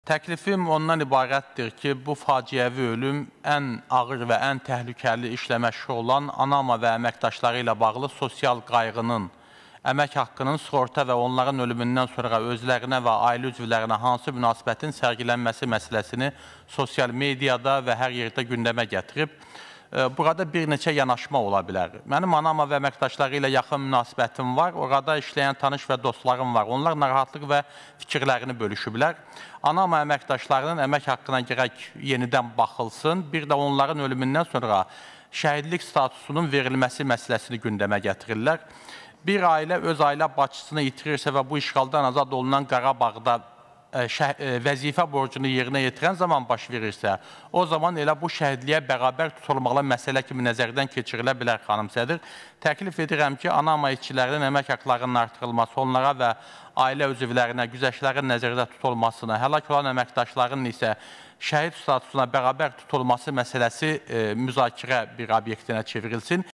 ANAMA - mina axtaran şəxslərin sosial qayğısı və onlara şəhidlik statusu verilməsi barədə
Razi Nurullayev, Milli Məclisin deputatı, Milli Cəbhə Partiyasının sədri